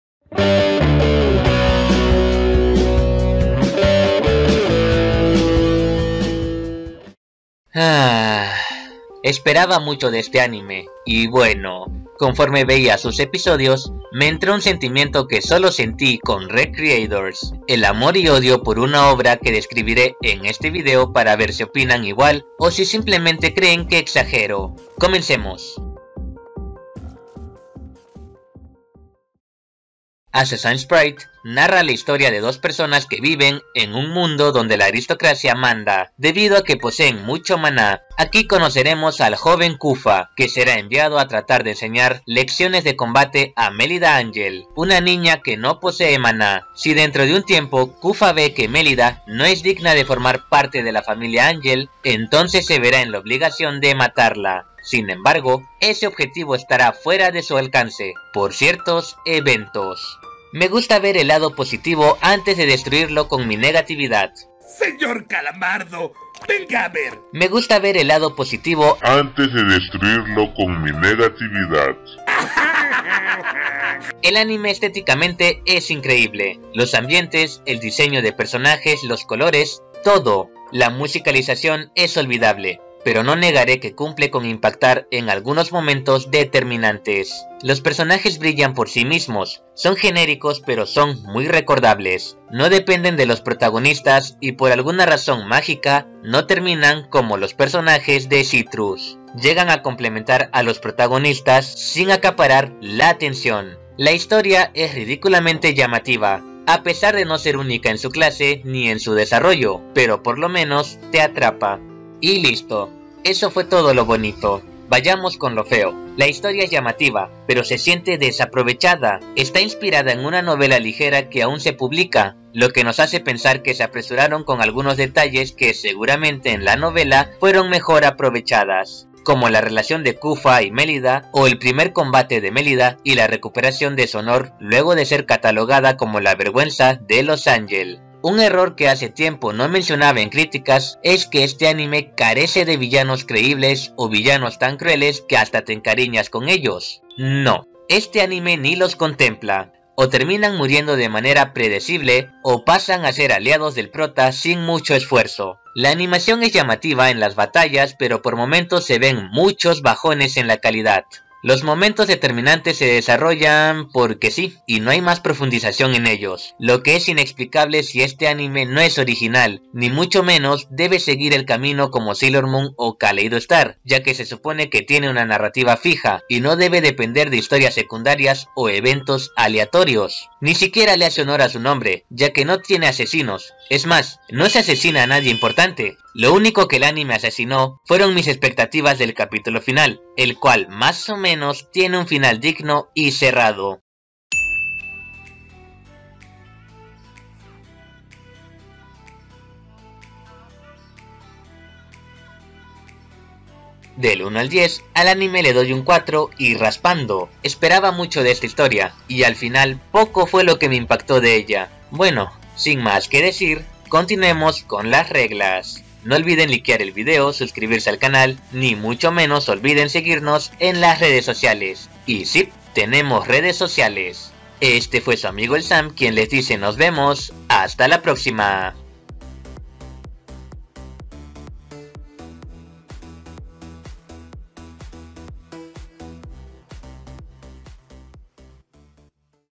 ASSASIN'S PRIDE: Otro potencial desperdiciado
Assasin's Pride es un anime que visualmente es impactante, su historia nos hacia creer que estábamos ante una de las sorpresas de la temporada. Pero la realidad fue otra y lamentablemente este anime es otro caso de potencial desperdiciado.

Assasin's Pride se encuentra disponible en Crunchyroll, no olviden verlo en esa plataforma para apoyar al anime legal.